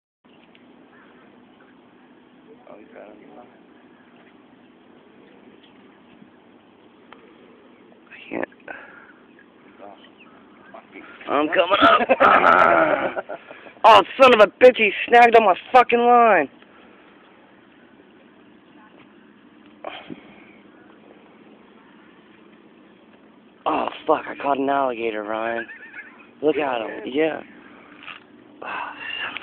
Alligator Snags Mike
A gator came to eat my legs, I jumped up from the dock but he had already snagged my line on his body.  Listen for the drag.